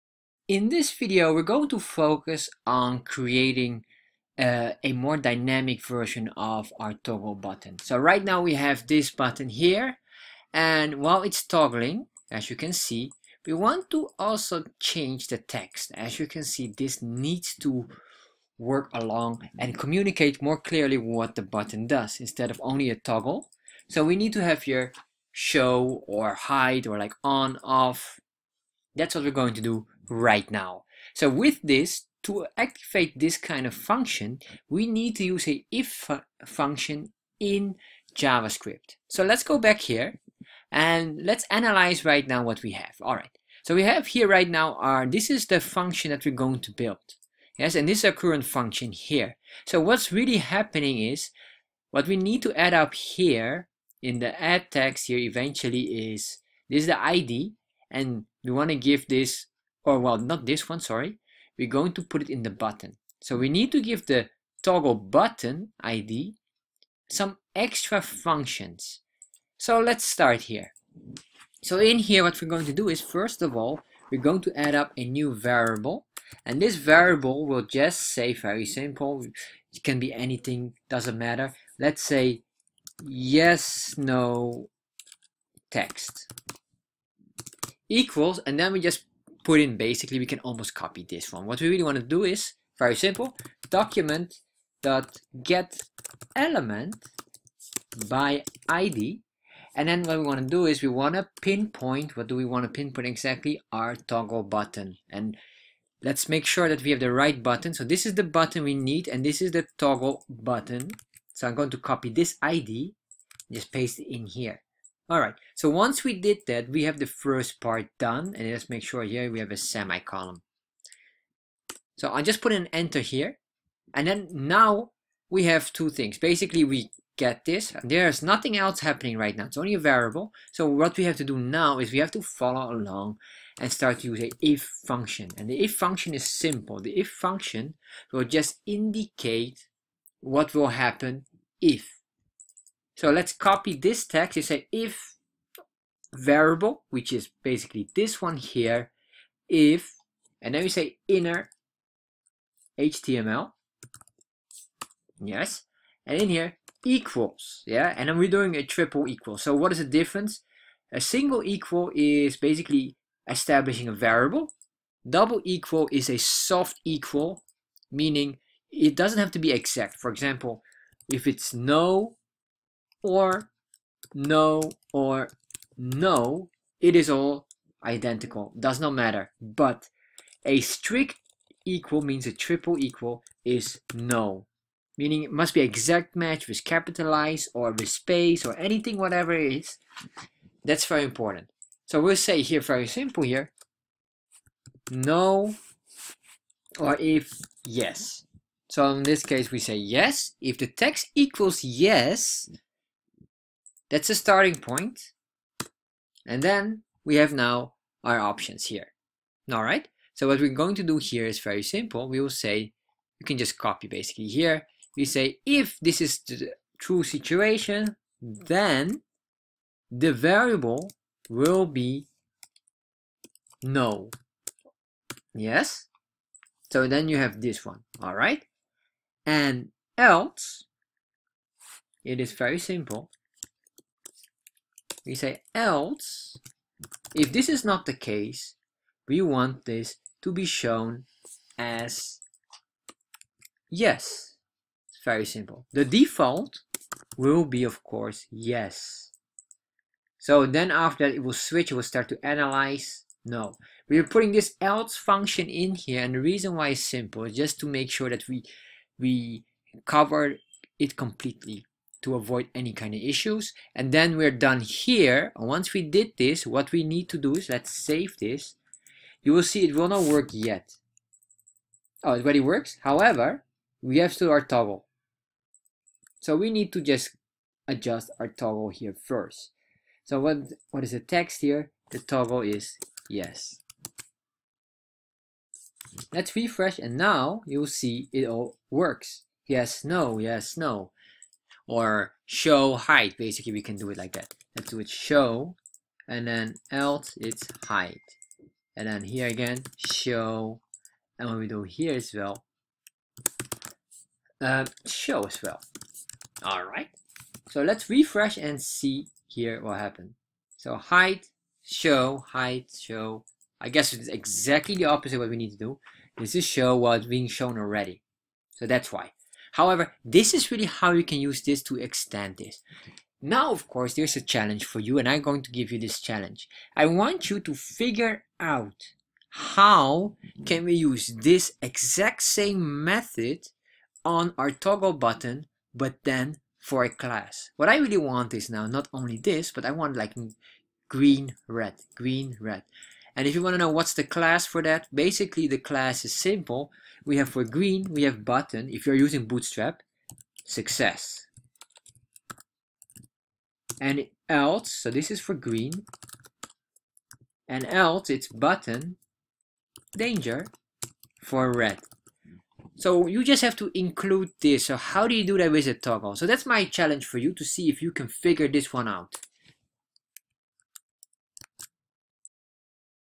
JS for beginners, toggle text in button [6/25]
Welcome to Excellence in Excel,

In this video you will learn in this video you will learn how toggle text in a button in JavaScript. Very often you want to show details or hide details the moment the user clicks a button. Making this higher level of interactivity is important.